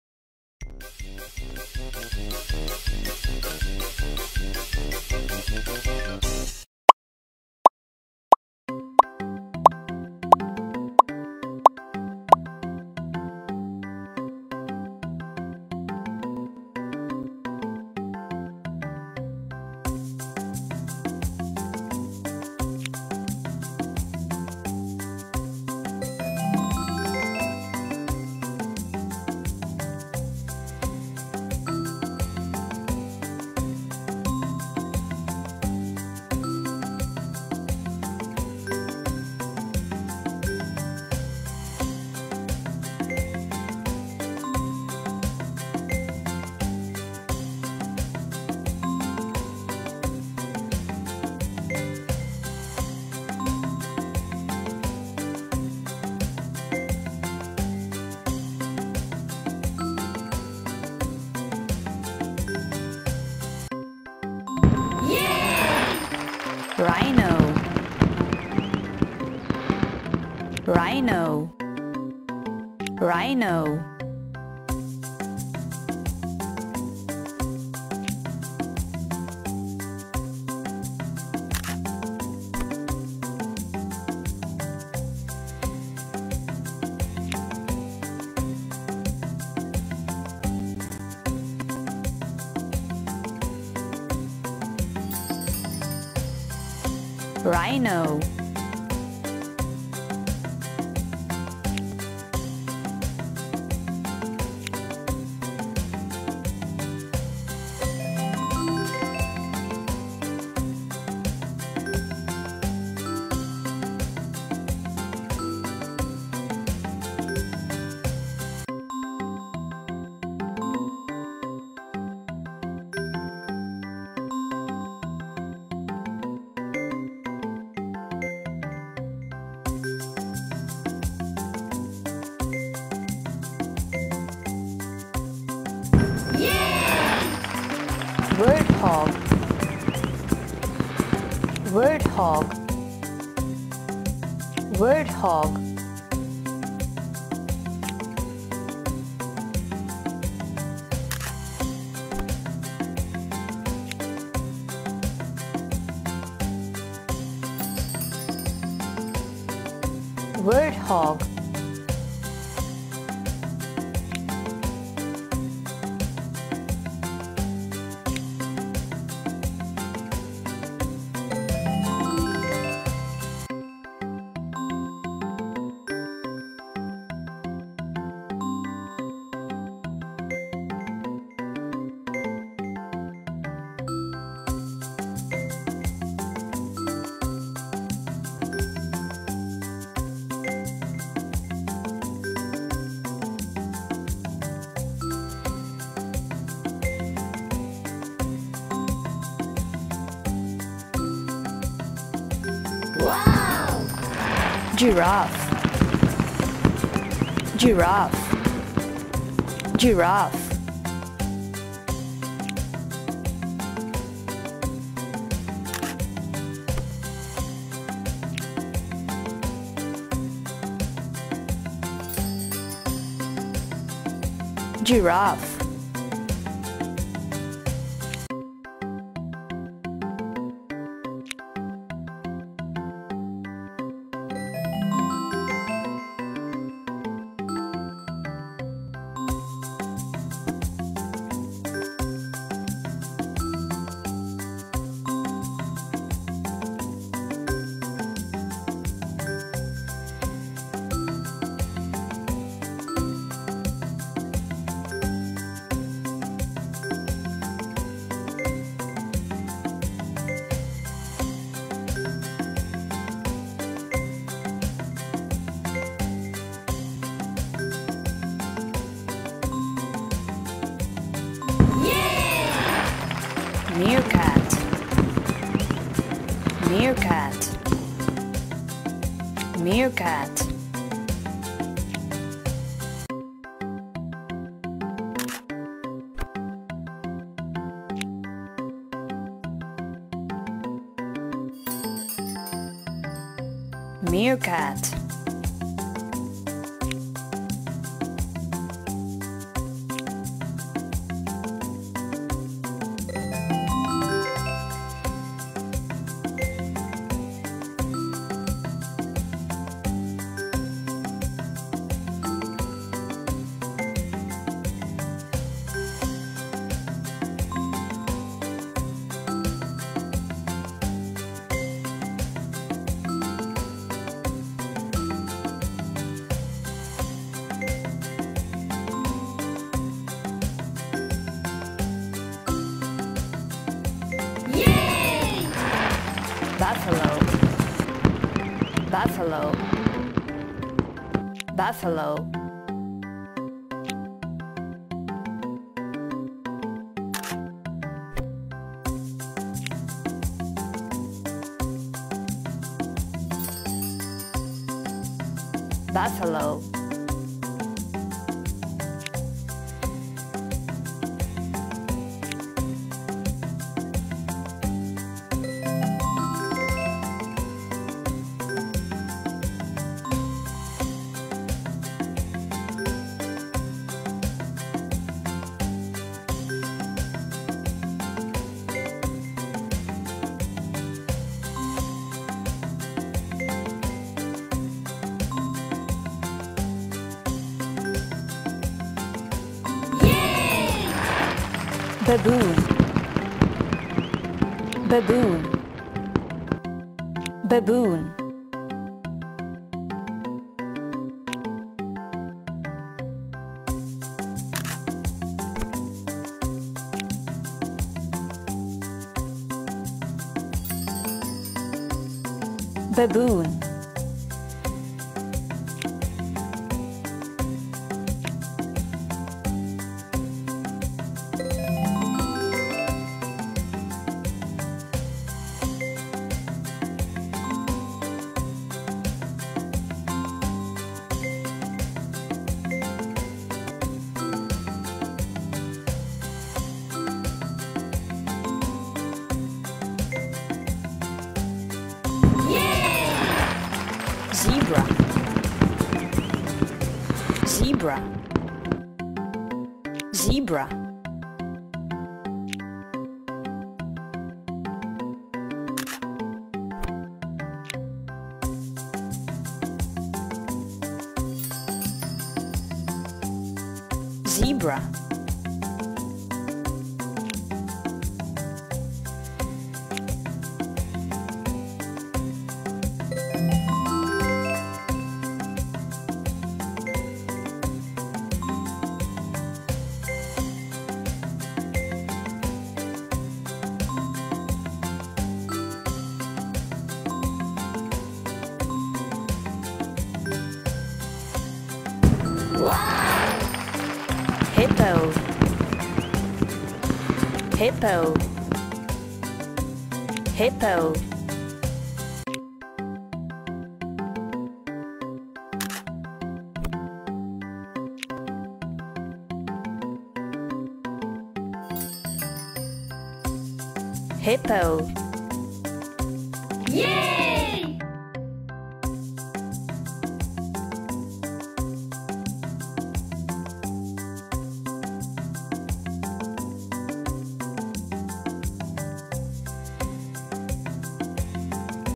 Learn African Animals Names For Kids In English (Part 1)
Learn African animals names with this interesting puzzle .
play and have fun and discover the names of 8 animals listed here :
1- Rhino
2-Warthog
3-Giraffe
4-Meerkat
5-Buffalo
6-Baboon
7-Zebra
8-Hippo
 listen and repeat.
Other More animals and Interesting videos are coming.